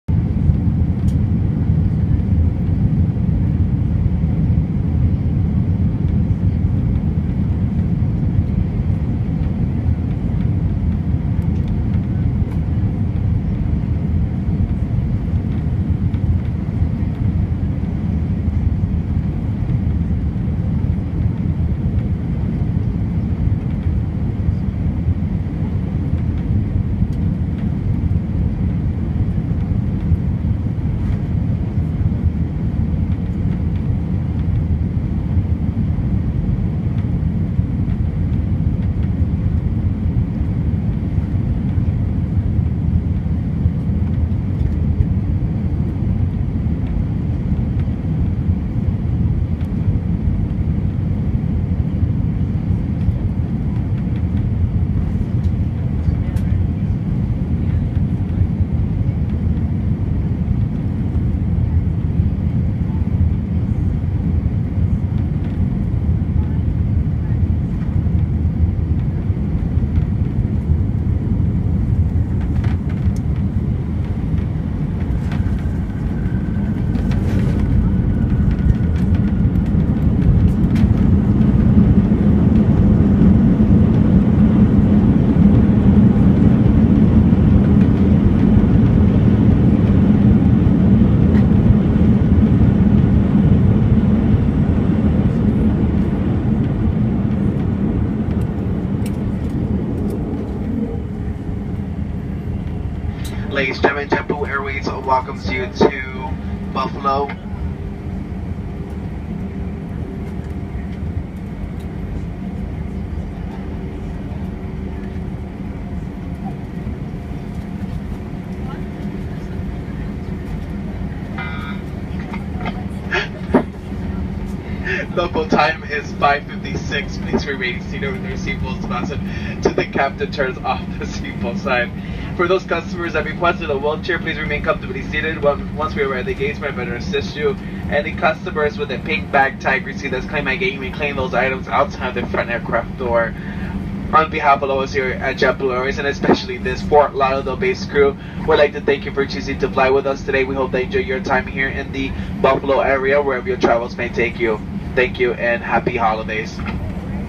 JetBlue Airbus A320 Landing at Buffalo/Niagara International Airport (BUF)
JetBlue Airbus A320 landing at Buffalo/Niagara International Airport (BUF) after a flight from Fort Lauderdale – Hollywood International Airport (FLL), on December 21, 2016.